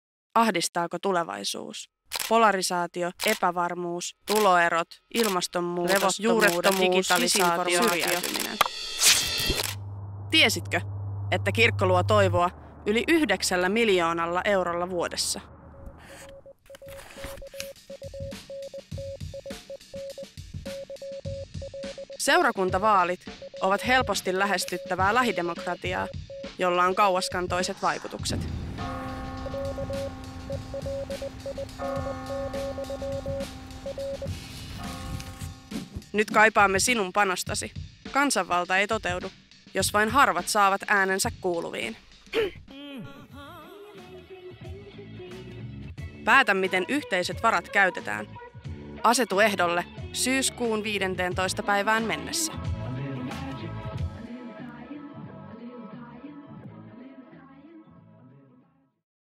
Seurakuntavaalit 2022
Huolestuttaako tulevaisuus?

Millaisia arvoja ja asioita haluaisit edistää seurakunnassasi?

Asetu ehdolle
Tulevaisuutta tehdään nyt